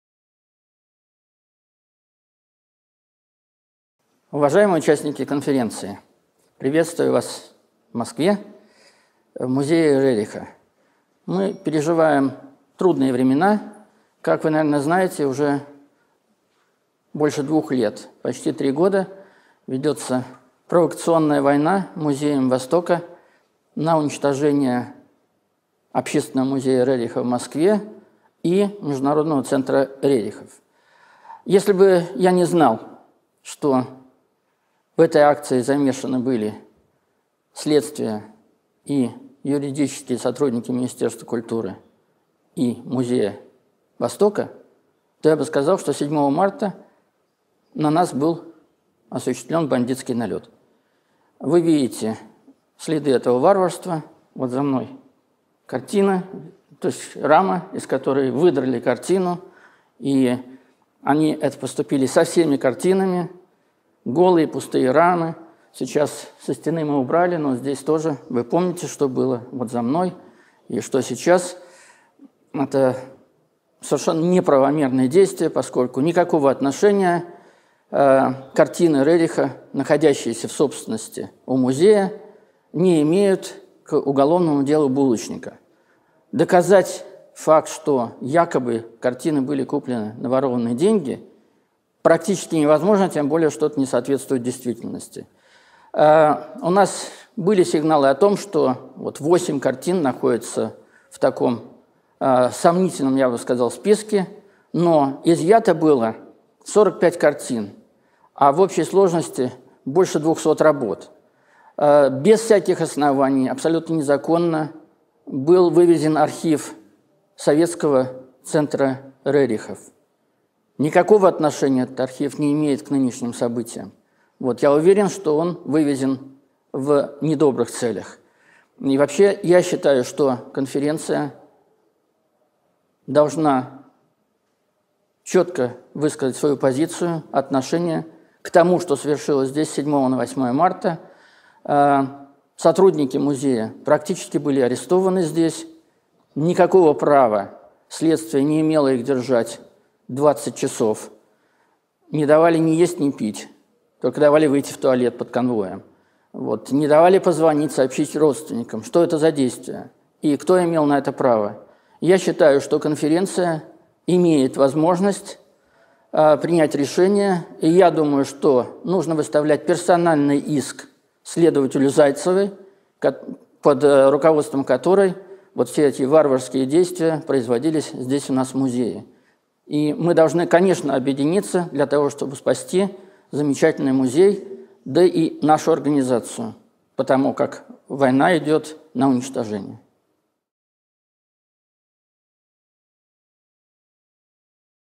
Анатолий Карпов: «Идет война на уничтожение Музея Рериха». 2017
Уважаемые участники конференции!
Приветствую вас в Москве, в Музее Рериха. Мы переживаем трудные времена: как вы, наверное, знаете, уже больше двух лет, почти три года, ведется провокационная война Музеем Востока на уничтожение общественного Музея Рериха в Москве и Международного Центра Рерихов.
Если бы я не знал, что в этой акции были замешаны следствие и юридические сотрудники Министерства культуры и Музея Востока, то я бы сказал, что 7 марта на нас был осуществлен бандитский налет. Вы видите следы этого варварства: вот за мной картина, то есть рама, из которой выдрали картину, и так они поступили со всеми картинами, остались голые пустые рамы. Сейчас мы их со стены убрали, но здесь, тоже вы помните, что было за мной и что сейчас – это совершенно неправомерные действия, поскольку никакого отношения картины Рериха, находящиеся в собственности у Музея не имеют к уголовному делу Булочника. Доказать факт, что картины якобы были куплены на ворованные деньги, практически невозможно, тем более, что это не соответствует действительности.
У нас были сигналы о том, что 8 картин находятся в таком сомнительном, я бы сказал, списке, но изъято было 45 картин, а в общей сложности – больше 200 работ. Без всяких оснований, абсолютно незаконно был вывезен архив Советского Фонда Рерихов. Никакого отношения этот архив не имеет к нынешним событиям. Я уверен, что он вывезен в недобрых целях, и вообще я считаю, что конференция должна четко высказать свою позицию, отношение к тому, что совершилось здесь с 7-го на 8-е марта. Сотрудники Музея были практически арестованы здесь, никакого права следствие не имело их держать 20 часов – не давали ни есть, ни пить, только давали выйти в туалет под конвоем, не давали позвонить, сообщить родственникам. Что это за действия, и кто имел на них право?
Я считаю, что конференция имеет возможность принять решение и, я думаю, что нужно выставлять персональный иск следователю Зайцевой, под руководством которой все эти варварские действия производились здесь, у нас в Музее.
И мы должны, конечно, объединиться, чтобы спасти замечательный Музей, да и нашу организацию, потому как война идет на уничтожение.

Сбор средств для восстановления культурной деятельности
общественного Музея имени Н.К. Рериха Международного Центра Рерихов